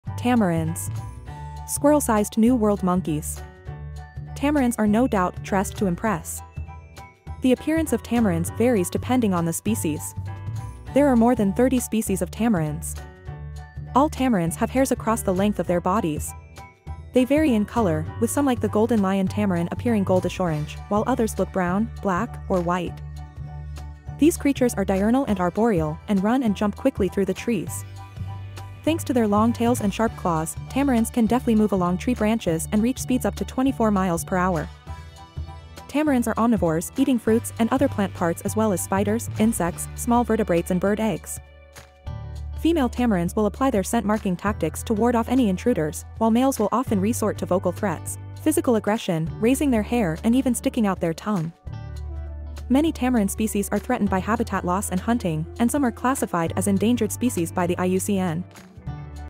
Tamarins - Squirrel Sized New World Monkeys 🐒 Tamarin Monkey Facts
Lets meet Tamarins - Squirrel Sized New World Monkeys

Recommended Product

➟ Dog Problem Behaviors Disappear!